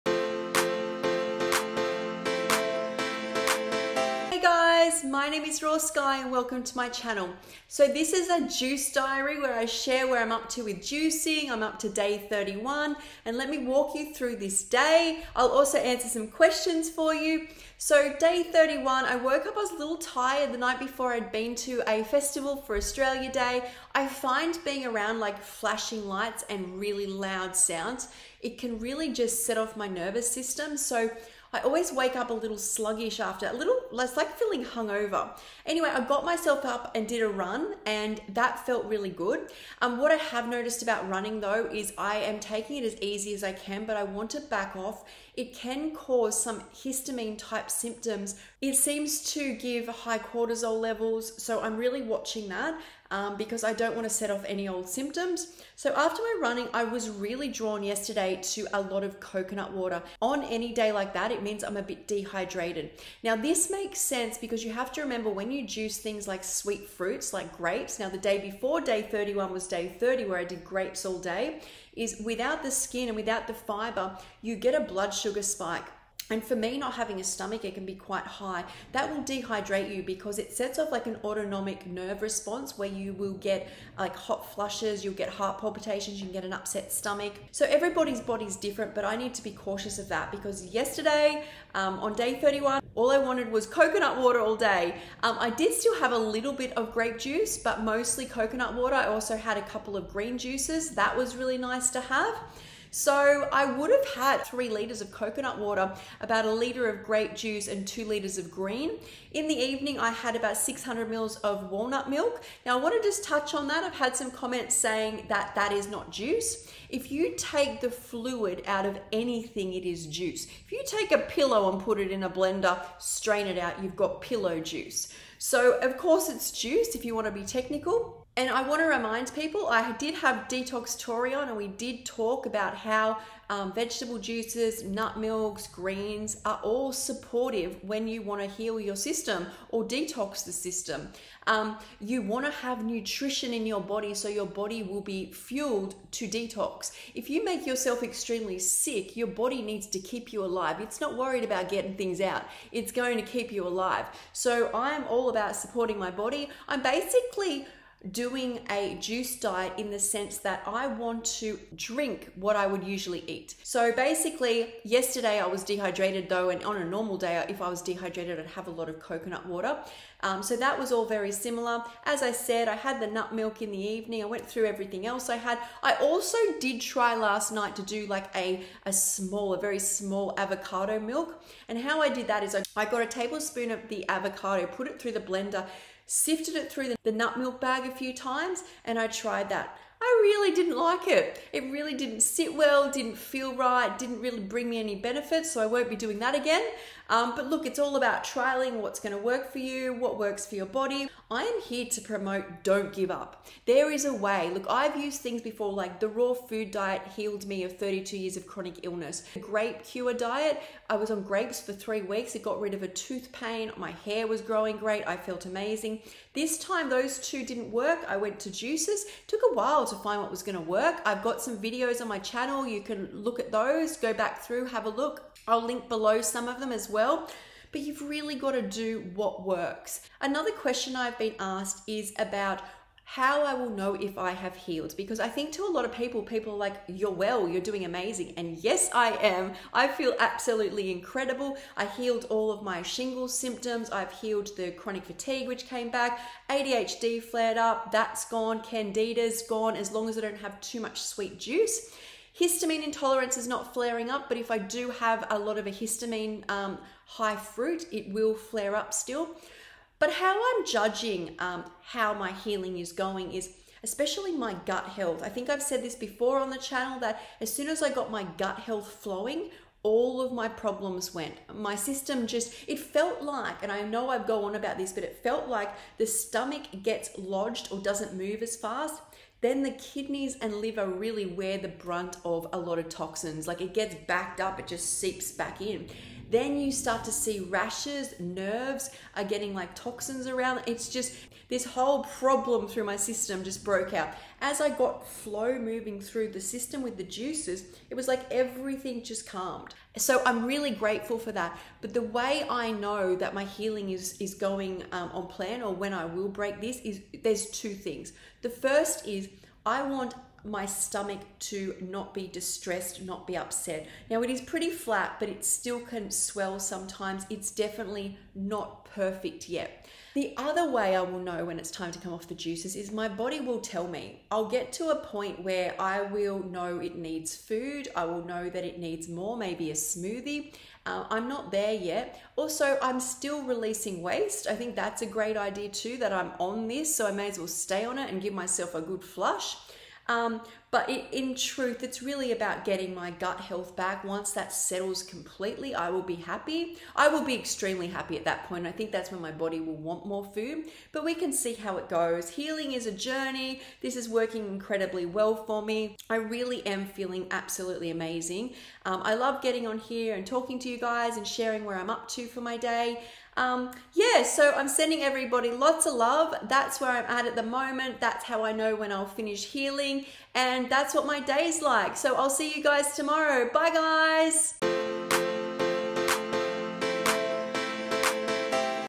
31 DAYS OF DRINKING ONLY JUICE!!! HOW WILL I KNOW WHEN TO STOP JUICING????
31 days drinking only juice to heal!!! I have healed so many symptoms so far and how will I know when to stop juicing??? I share the answer to this today in this video!

Since juicing  healed symptoms of:

Shingles complications
Postherpetic neuralgia (PHN)
Dizziness
POTS
Chronic tiredness,
Unexplained rashes
Heightened immune response
Histamine intolerance
Candida
Brain fog
Anxiety
Depression
ADHD